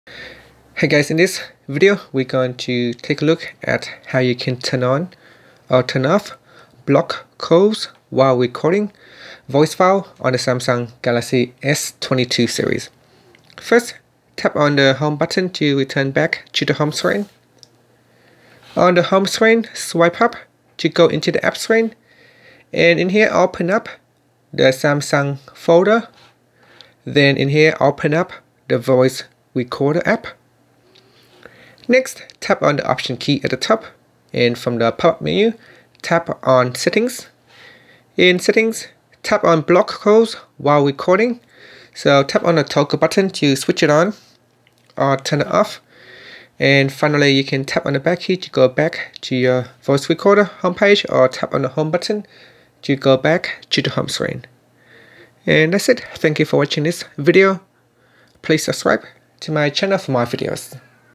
Galaxy S22/S22+/Ultra: How to Turn On/Off Block Calls While Recording For Voice Recorder
Learn how you can turn On/Off Block Calls While Recording for Voice Recorder on the Samsung Galaxy S22/S22+/Ultra.